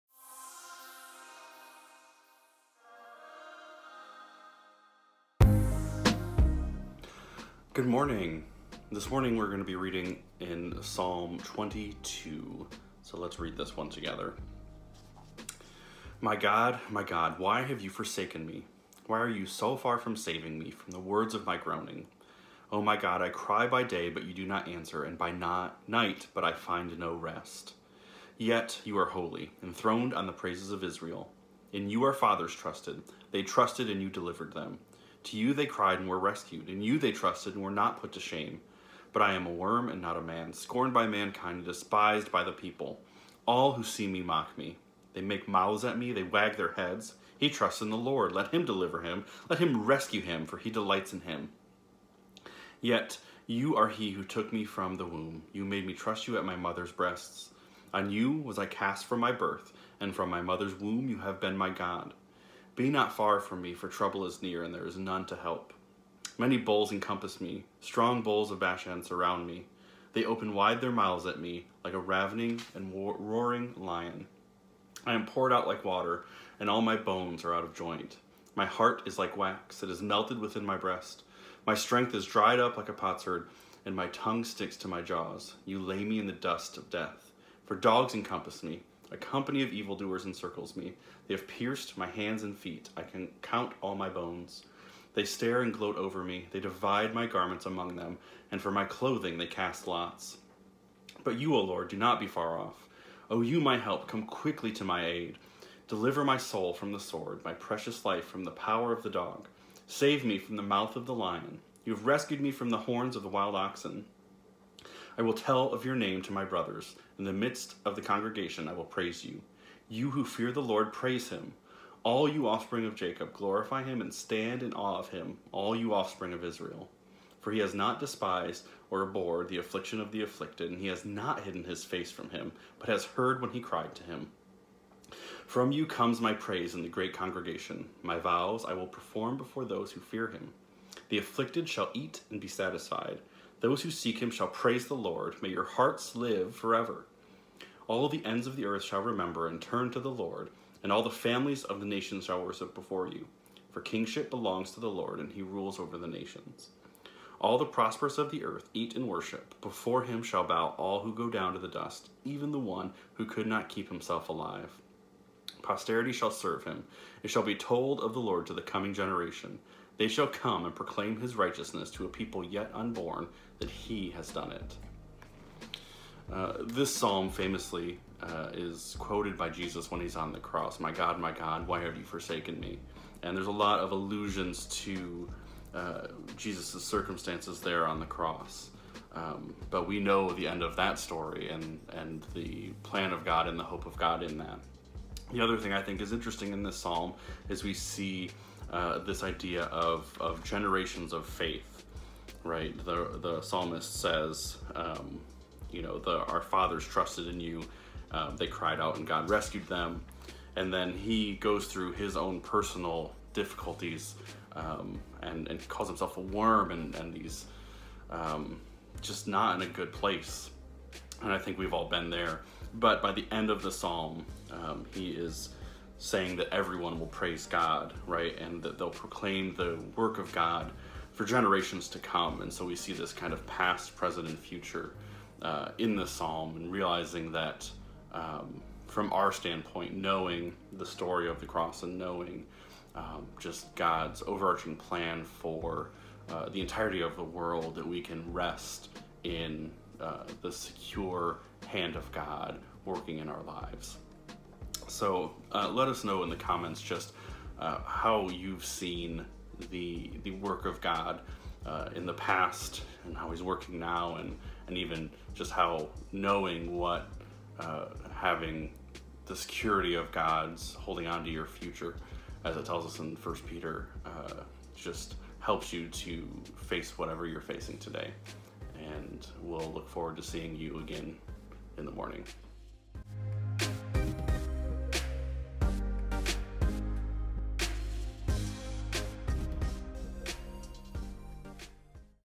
Psalm 22 Devotional
Our Elders are leading us in devotions through the Psalms. Read along and pray with us as we look at Psalm 22 today.